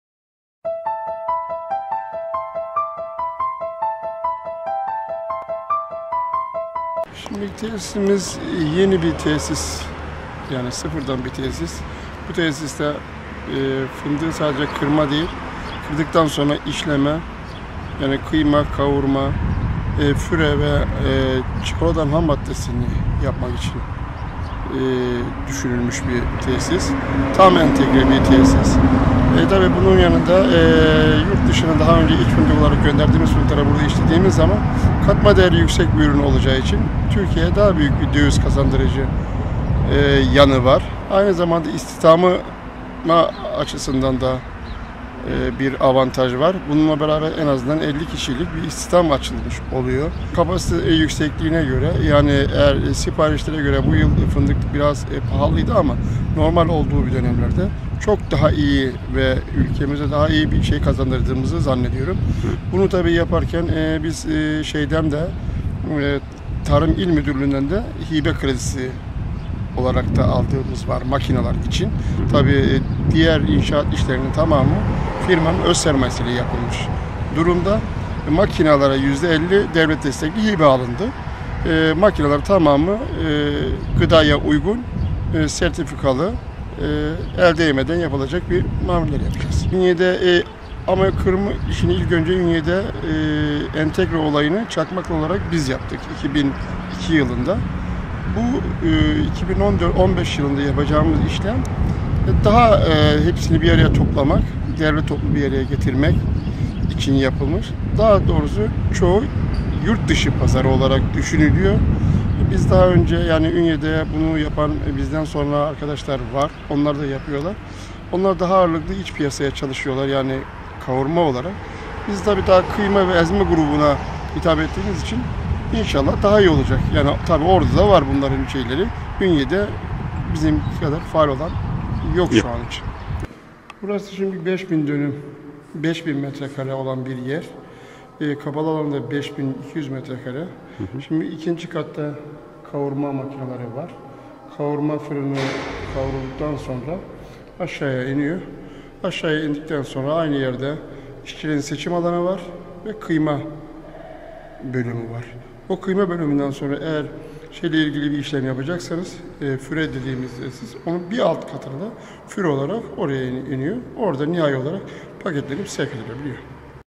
Çakmaklar Fındık Ünye’ye devasa tesis kazandırdı
Karadeniz’in en değerli ürünü olan fındığı işlenmesinde kırma, işleme, kıyma, kavurma, füre ve çikolatanın hammaddesi yapmak için düşünülmüş çok sayıda kişi istihdamı sağlayacak büyük tesis Ünye’ye hatta ülkeye kazandırılıyor.